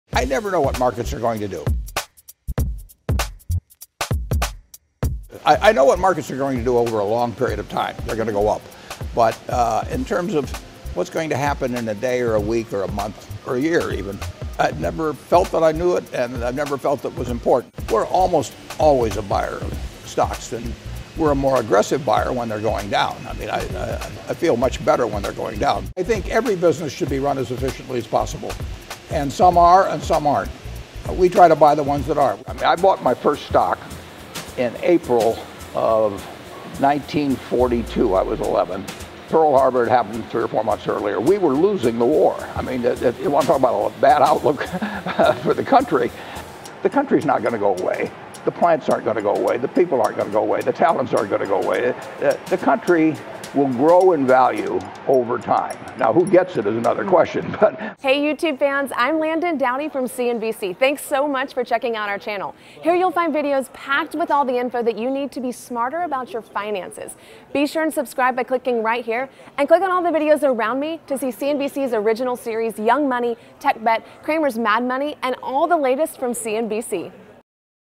Warren Buffett's Five Tips For Long-Term Investing | CNBC
Berkshire Hathaway CEO and iconic investor Warren Buffett shares five pieces of wisdom on how to adopt his long-term investing strategy.